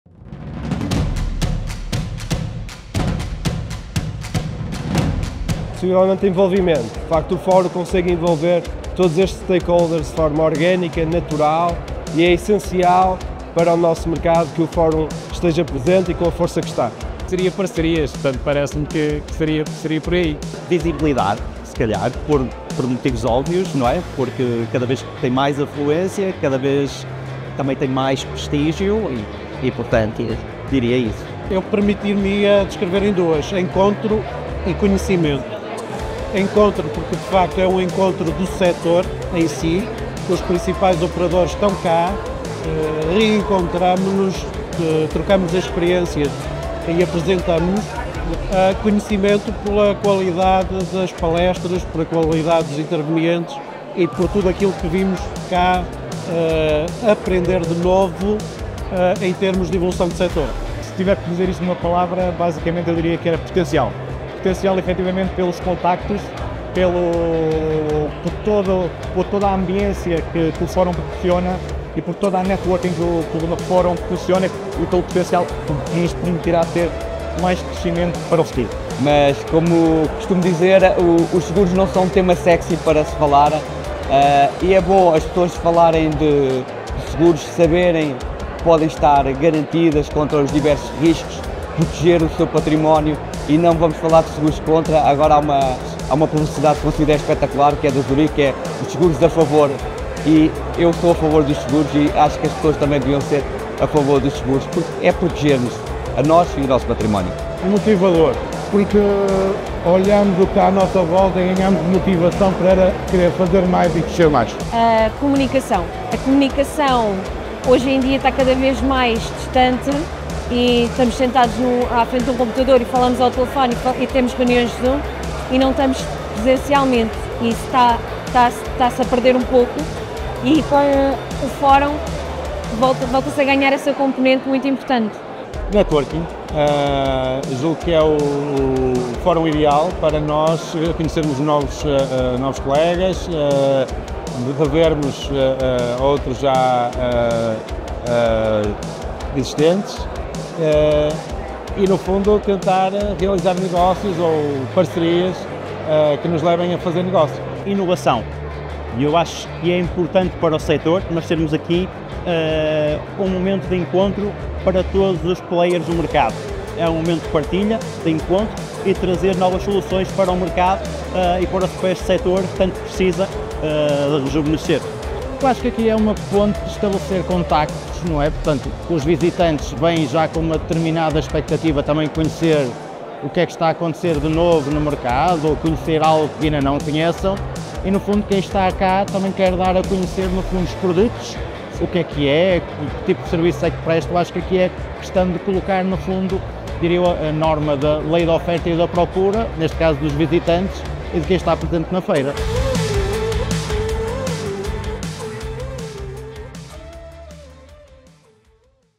Entrevistas Fórum Nacional de Seguros
O Fórum Nacional de Seguros, uma iniciativa do ECO, decorreu nos dias 2 e 3 de julho, no Porto.